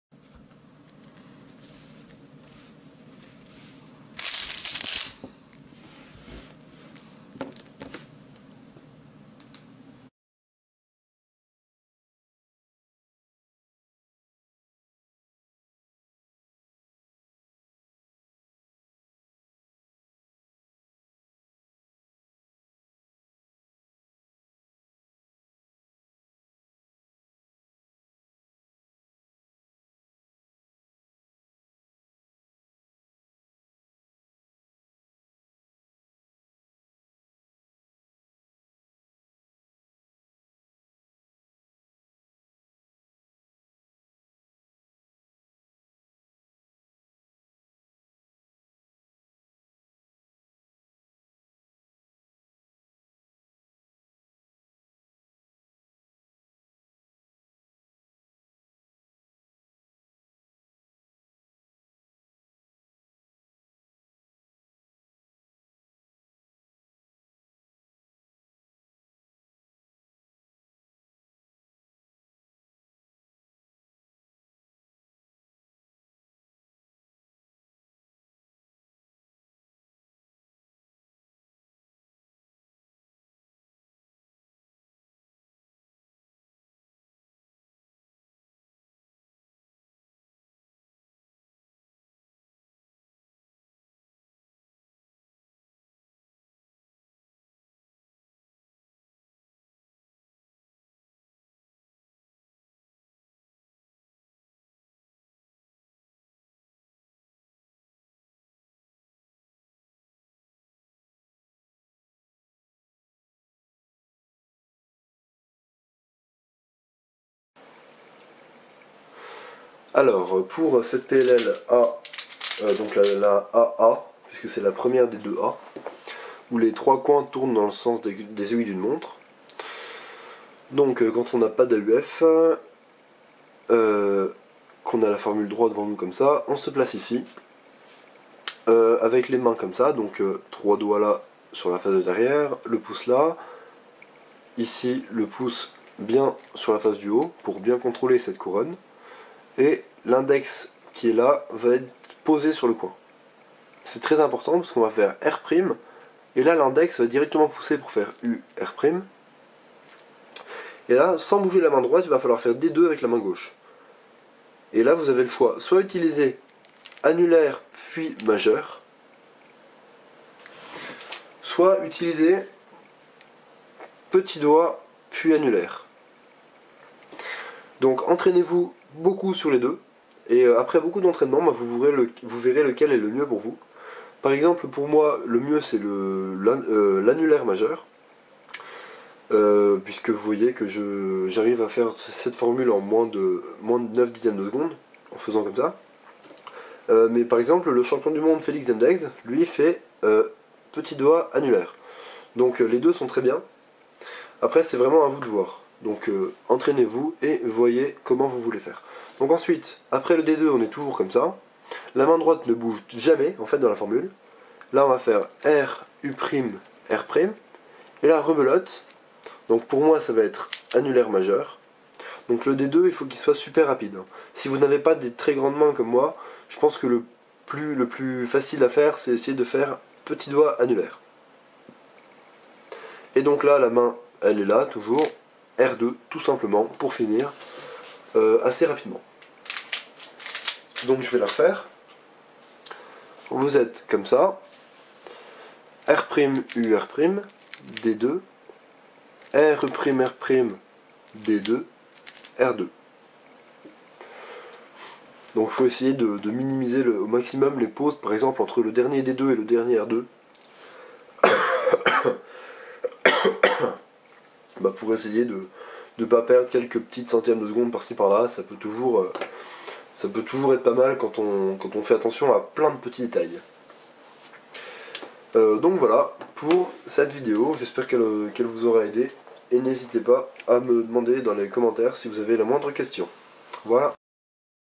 A perm (a) : 0,85sec [slow motion] + mes fingertricks détaillés
Vous pouvez me rejoindre sur ma page Facebook !

La formule : x R' U R' D2 R U' R' D2 R2